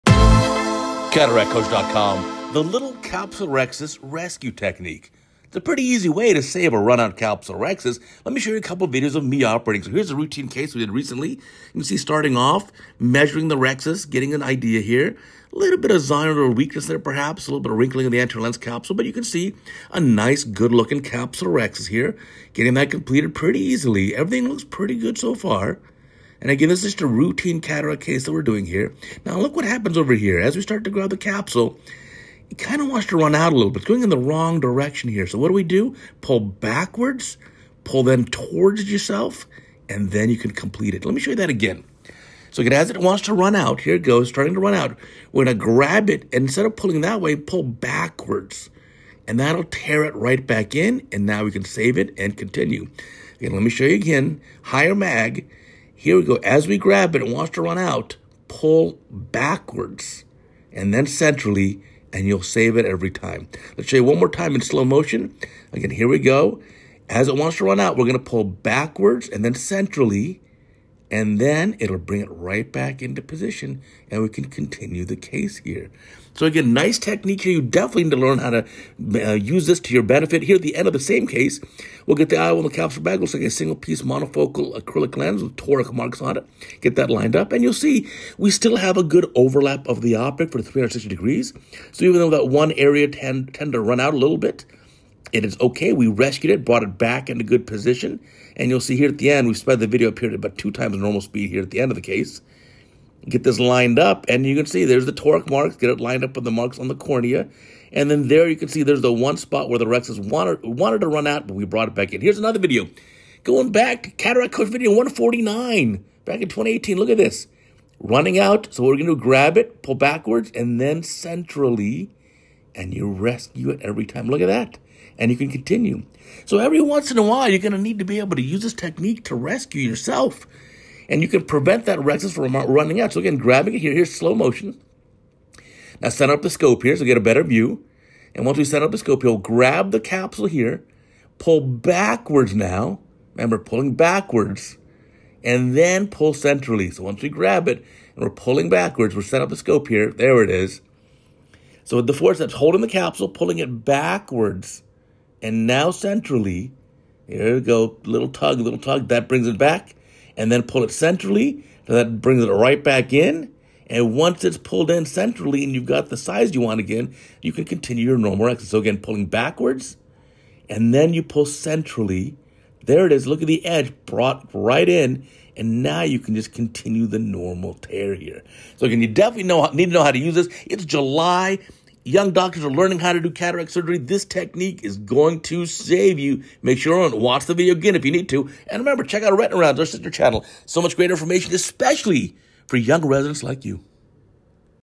CataractCoach™ 2633: The Little Capsulorhexis Rescue Technique
This technique is absolutely critical and it is something that every cataract surgeon must master. It allows us to rescue a run-out capsulorhexis and safely complete it without further complications. It is July and we are at the beginning of the academic here in the USA (and in many other countries particularly in the northern hemisphere) and that means our residents are at the early part in the learning curve. Do yourself a favor and make sure that you master this technique because it will save you, just like it did for me recently.